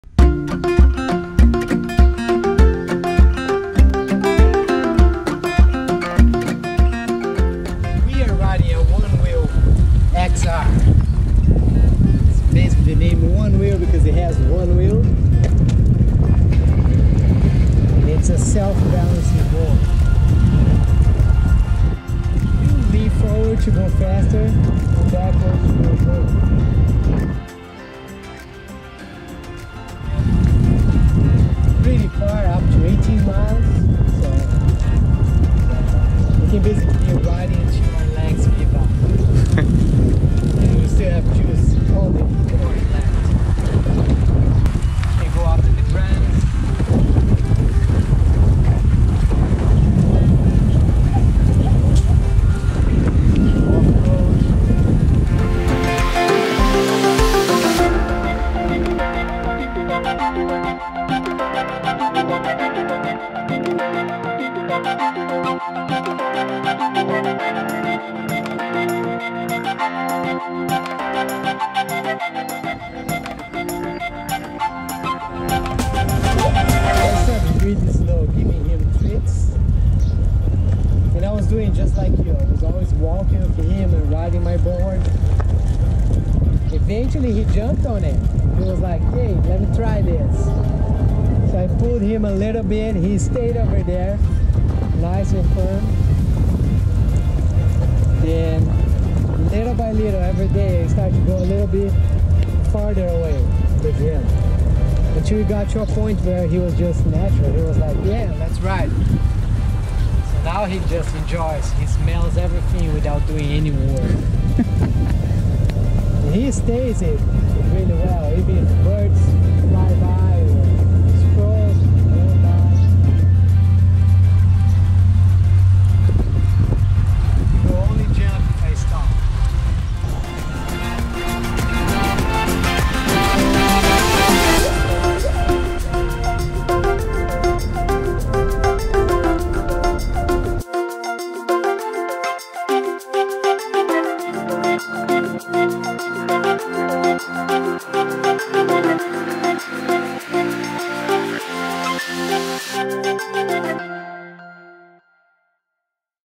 Onewheel and the Dog
A story about a Onewheel and the dog who rides it.

Filmed using a GoPro Hero 5 with Karma Grip, DJI Mavic Air, and iPhone X